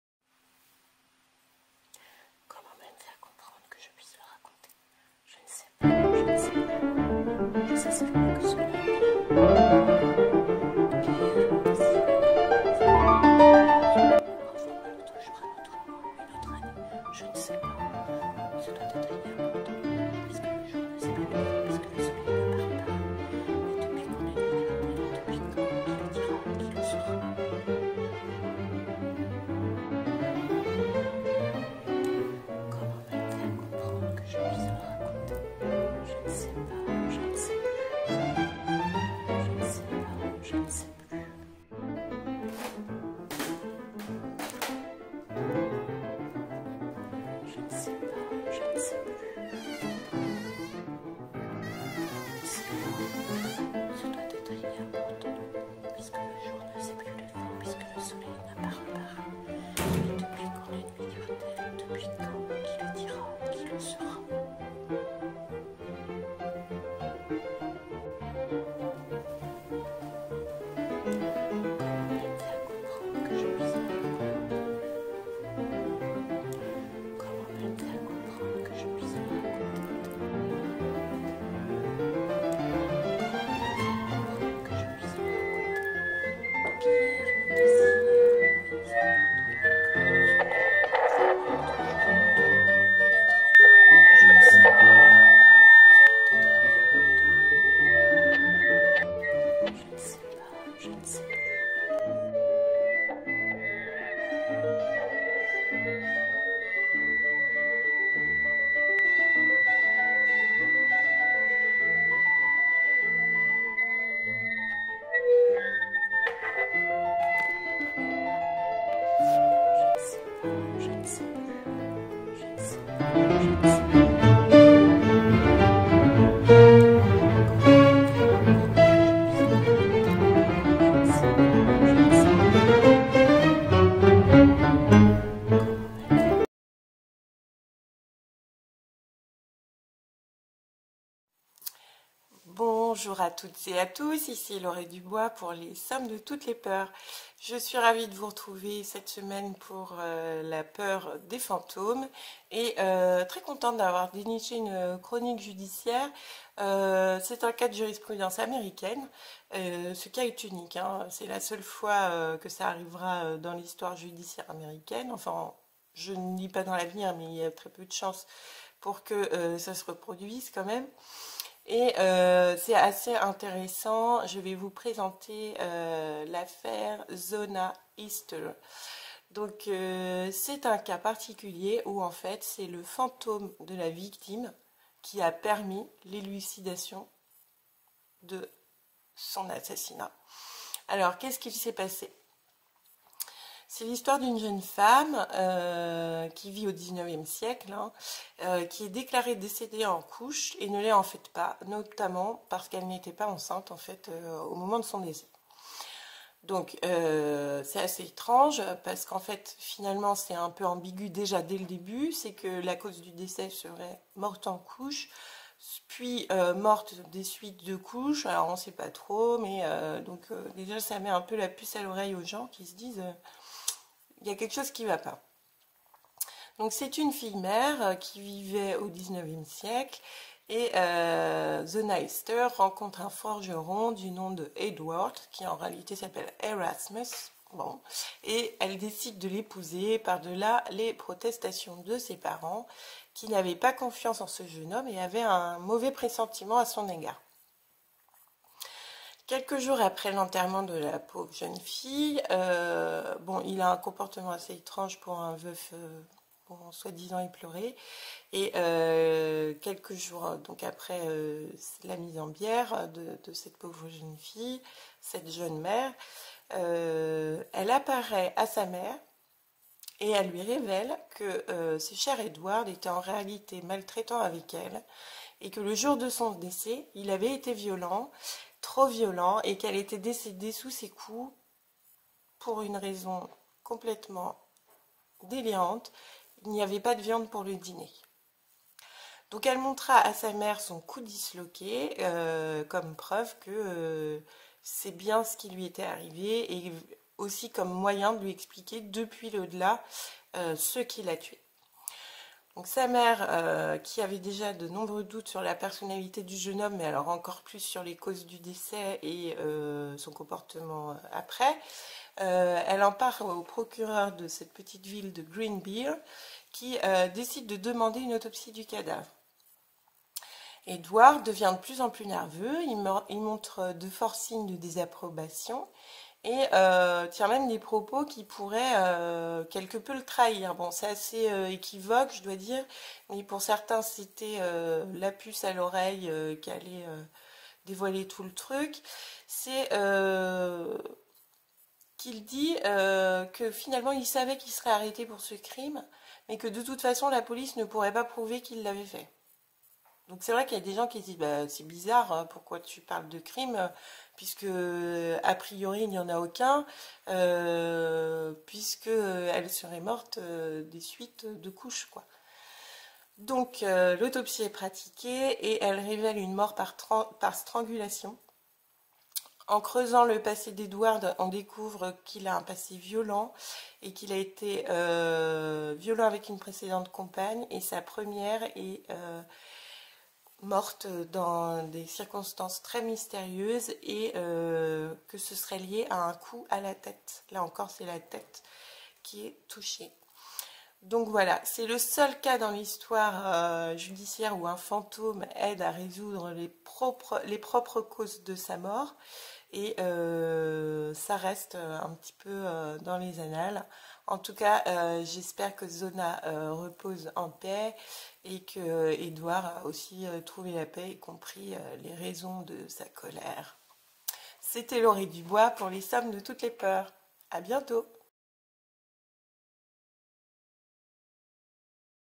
Mort suspecte #8 affaire Zona Heaster
Une mort pas si suspecte que cela, 1er et unique cas de Jurisprudence américaine où le fantôme aide à la résolution d'une affaire.
Pour les références cf les références de la semaine dans la Peur KEZAKO#8.